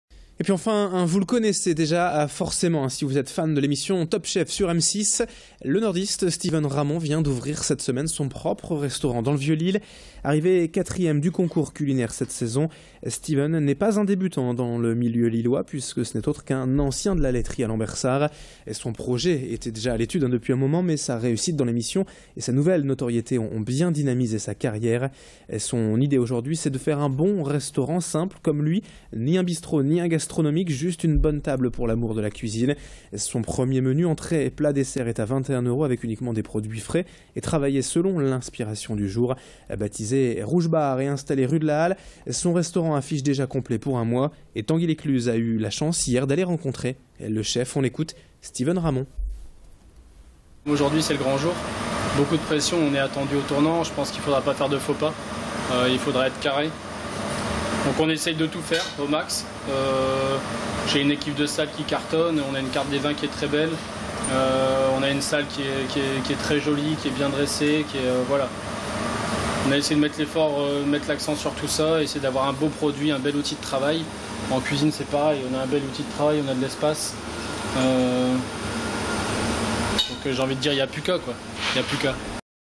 Jour J pour Steven Ramon!
Steven Ramon, un ancien candidat de Top Chef, a ouvert son propre restaurant dans le Vieux-Lille.

Sites webs/blogs, copiez cette vidéo avec le bouton "Intégrer" !